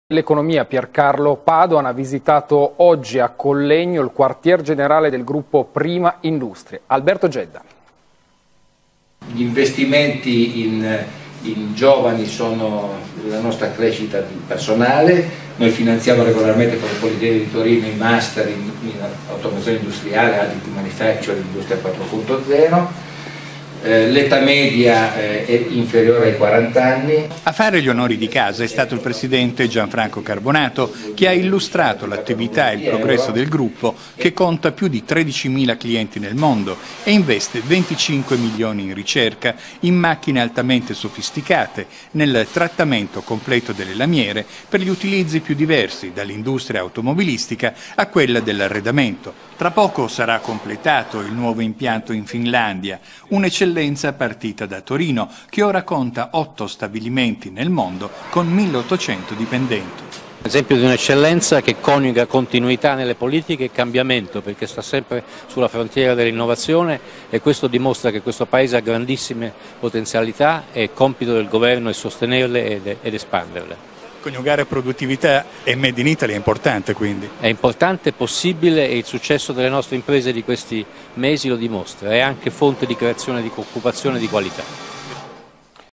Prima Industrie - Visita Ministro Padoan
Il servizio sulla visita del ministro dell'economia Pier Carlo Padoan all'HQTC (Headquarters and Technology Center) di Prima Industrie e Prima Power, andato in onda nell'edizione del TGR Piemonte del 08/02/2018